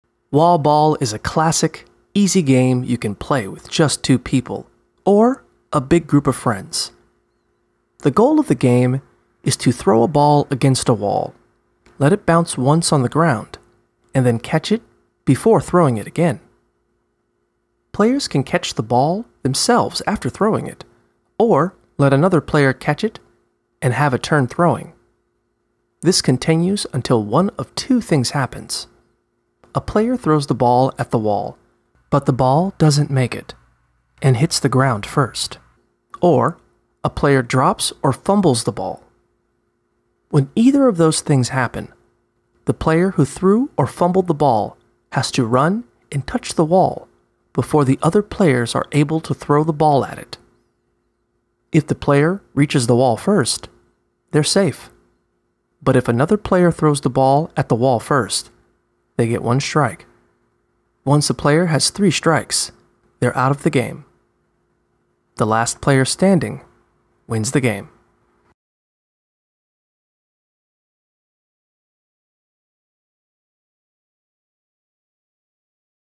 How to Play Wall Ball
Learn how to play wall ball with this guide from wikiHow